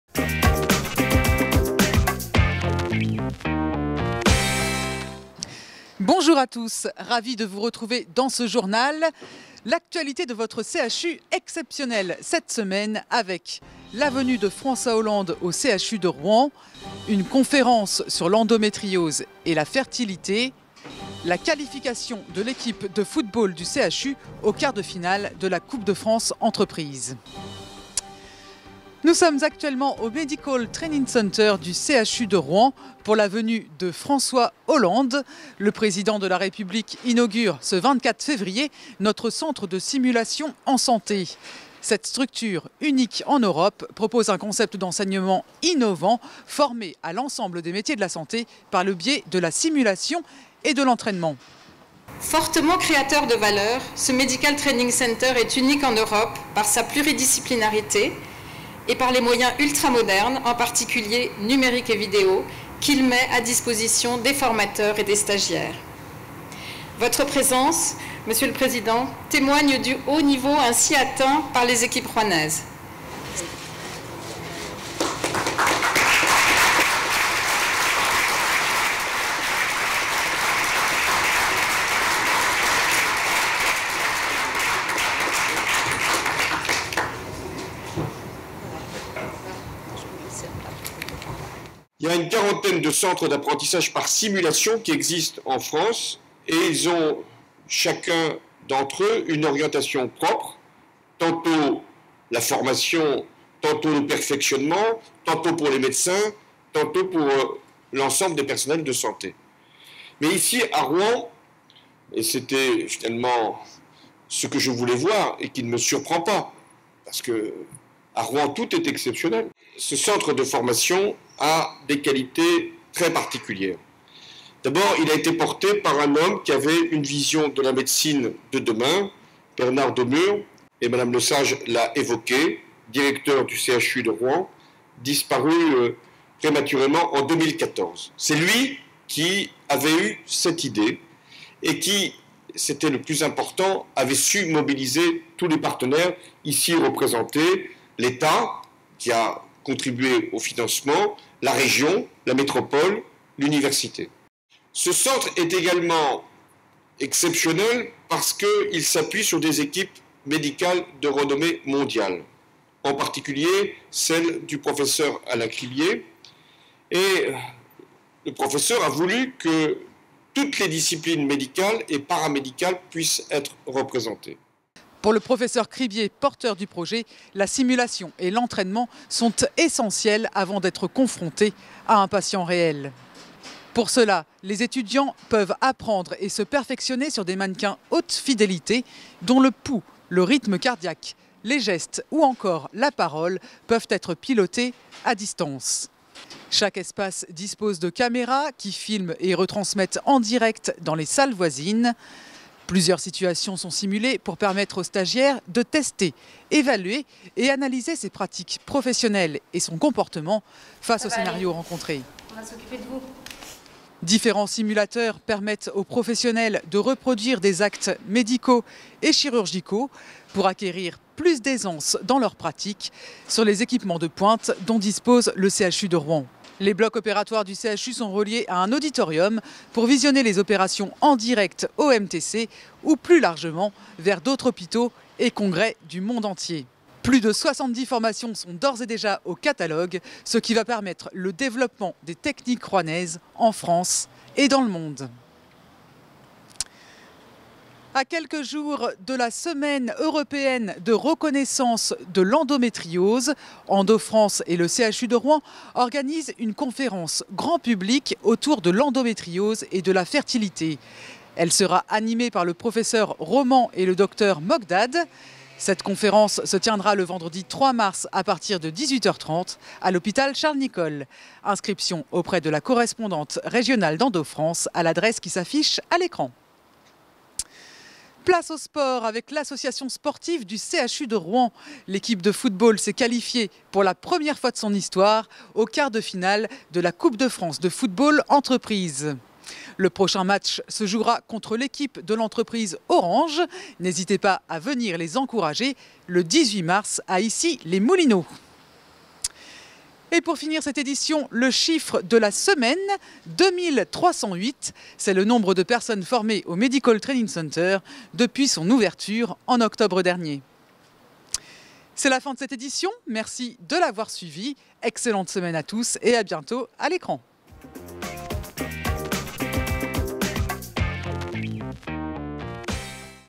Infos du 27 Février au 5 mars
- La venue de François Hollande au CHU de Rouen,
- Conférence "Endométriose et fertilité",
- L'équipe de football du CHU en 1/4 de finale de la Coupe de France Entreprise !

Journaliste : Mathilde LAPOTRE
Images et son : Christophe DUVAL
Montage : Jennifer LIOT